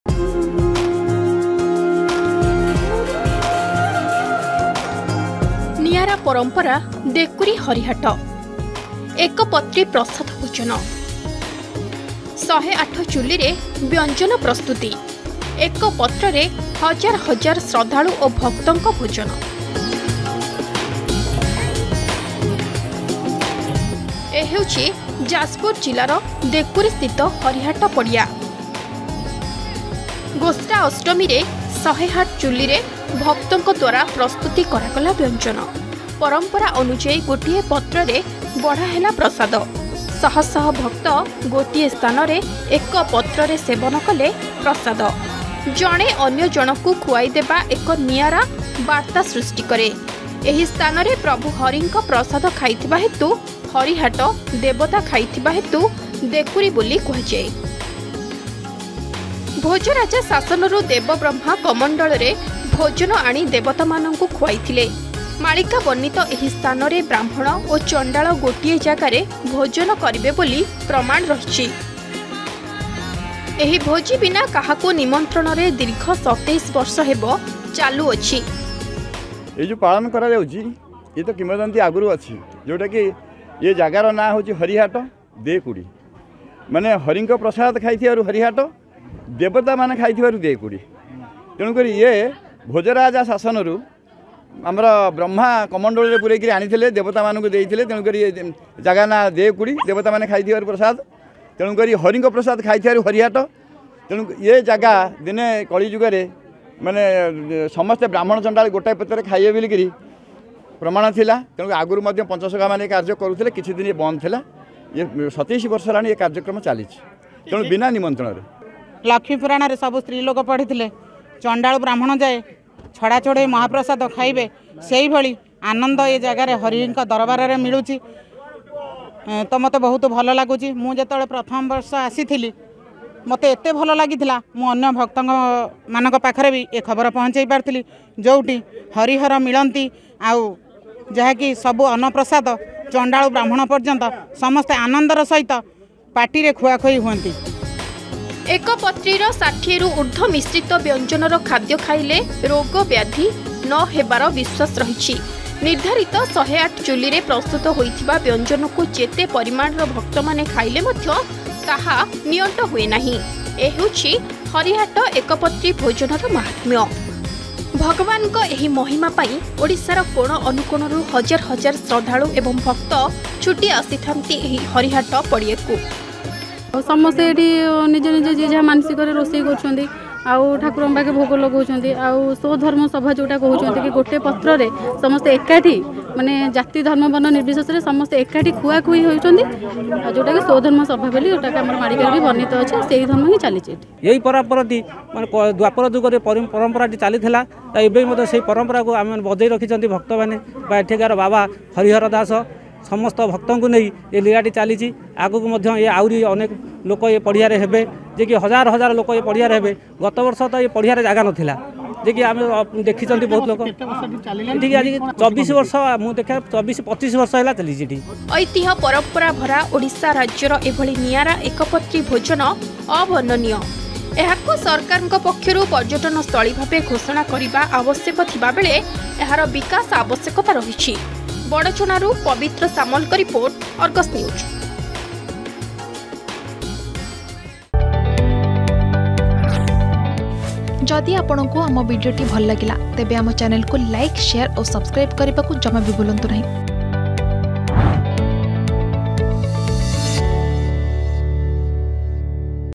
ନିଆରା ପରମ୍ପରା ଦେକୁରୀ ହରିହାଟ | Festival | Jajpur | Argus News
ଏହା ଯାଜପୁର ଜିଲ୍ଲାର ଦେକୁରୀ ସ୍ଥିତ ହରିହାଟ ପଡ଼ିଆ । ଗୋଷ୍ଟା ଅଷ୍ଟମୀରେ ୧୦୮ ଚୂଲିରେ ଭକ୍ତଙ୍କ ଦ୍ବାରା ପ୍ରସ୍ତୁତ କରାଗଲା ବ୍ୟଞ୍ଜନ । ପରମ୍ପରା ଅନୁଯାୟୀ ଗୋଟିଏ ପତ୍ରରେ ବଢ଼ା ହେଲା ପ୍ରସାଦ । ଶହ ଶହ ଭକ୍ତ ଗୋଟିଏ ସ୍ଥାନରେ ଏକ ପତ୍ରରେ ସେବନ କଲେ ପ୍ରସାଦ।